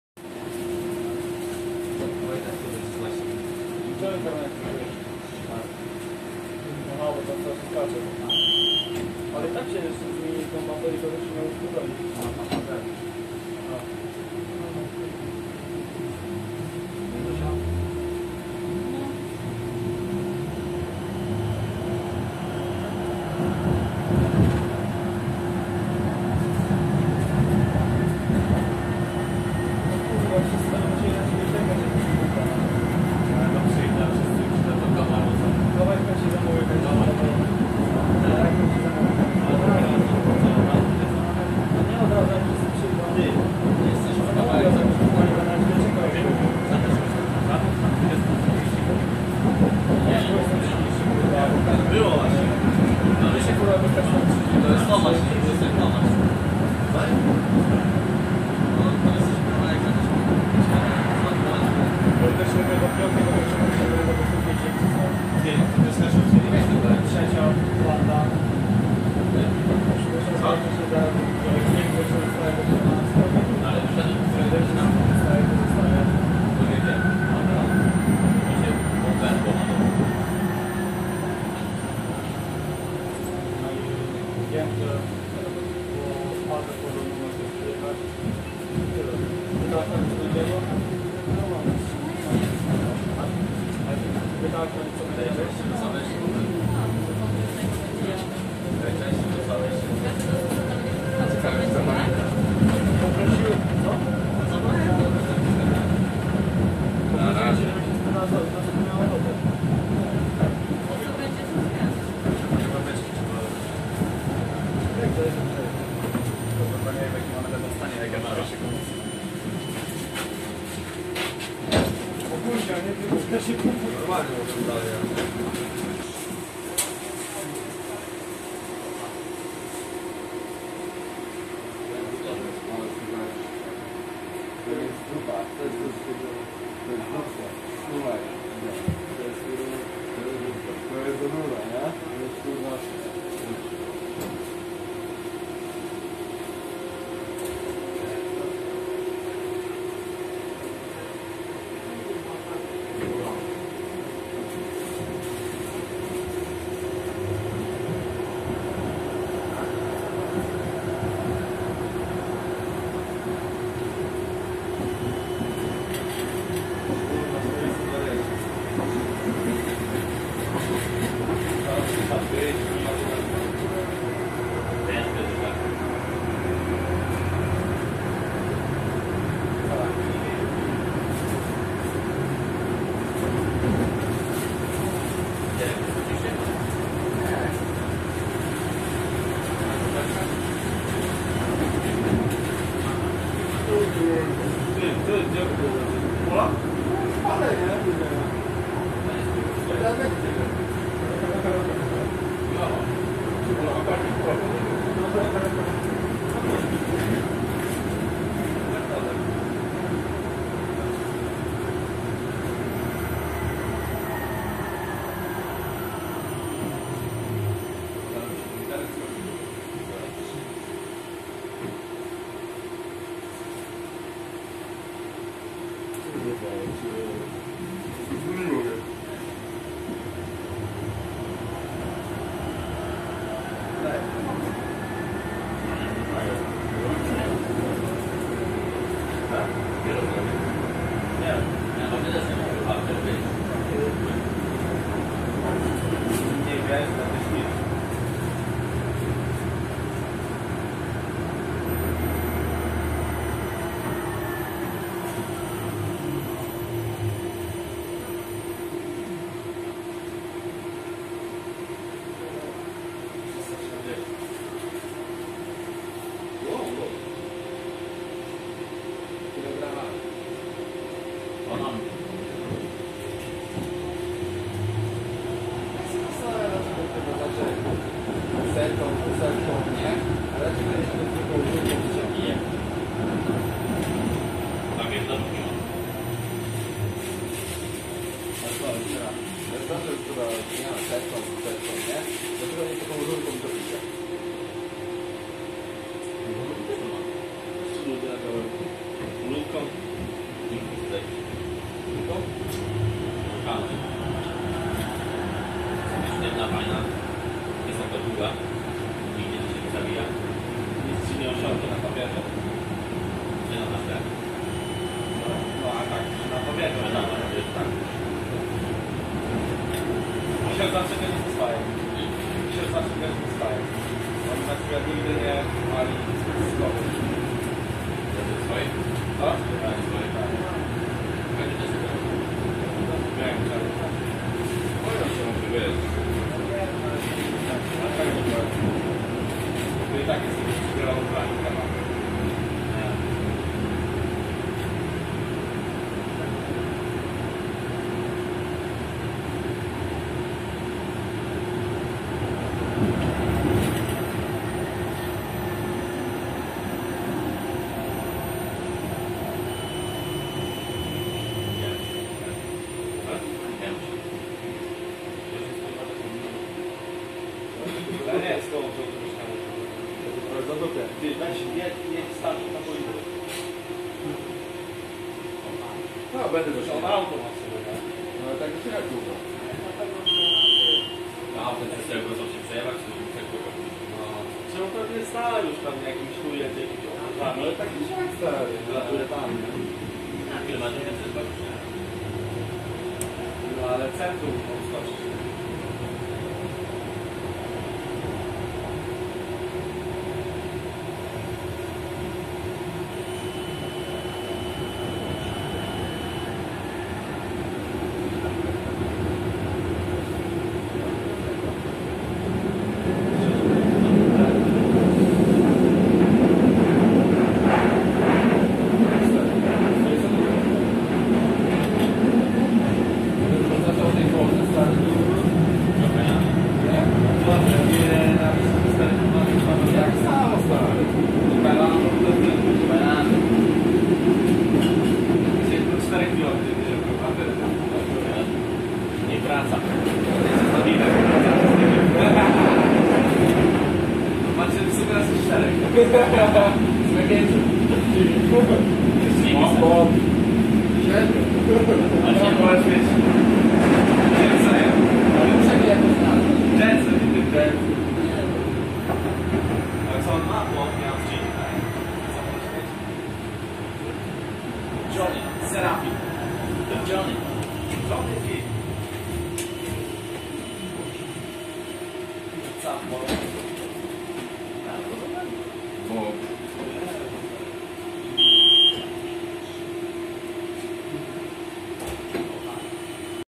[MPK Wrocław] Protram 205WrAs #2703 /[23] kier. Kowale\
205WrAs jeszcze przed modernizacją, znakiem rozpoznawczym #2703 jest Krasnal umiejscowiony na dachu, tuż nad kabiną motorniczego, jak sie dalej sytuacja z nim potoczy - niewiadomo

Przystanki:
1. Mosty Warszawskie
2. Kromera
3. Kętrzyńska
4. Kwidzyńska
5. Gęsia
6. Bociania